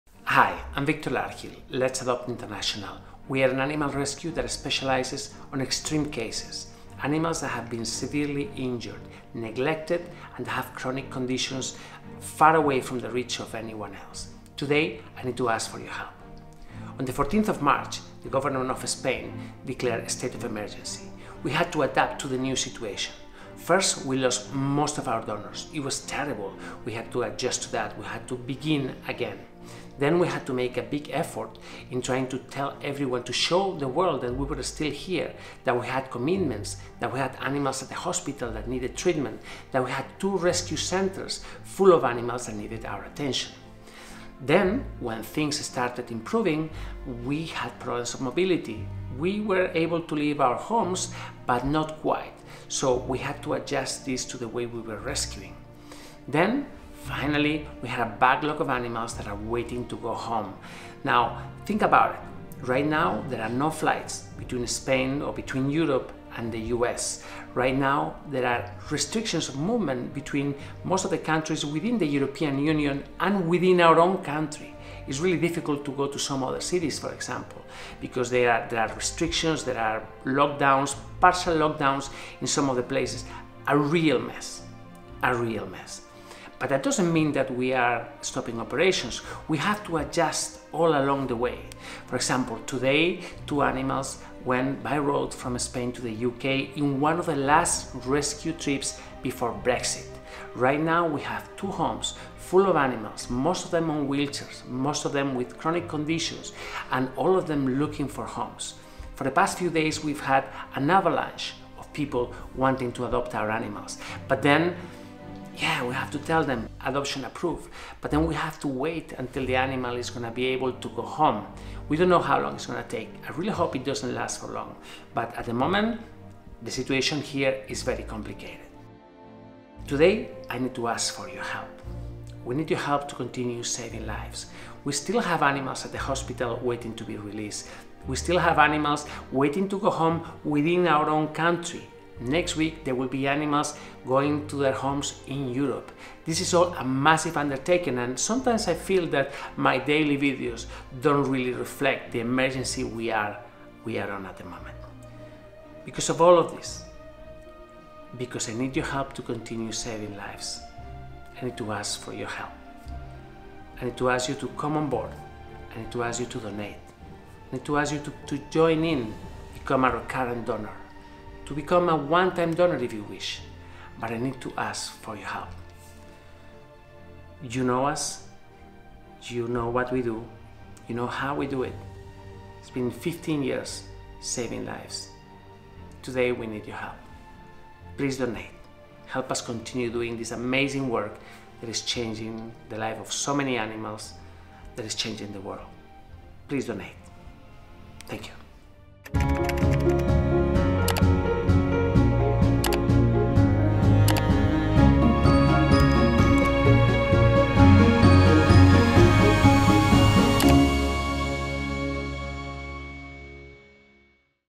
EMERGENCY! WE NEED YOUR HELP... OUR ANIMALS ARE STUCK!
Hi.. we have confirmed several adoptions over the last few weeks, and
are making a major effort to get these animals home, but… the
uncertainty derived from the pandemic is making this difficult… we are
nearly at a standstill. At the same time, our two rescue centers are
full with our long-term residents and we have animals undergoing
treatment at the hospital…
We need your help to get through this...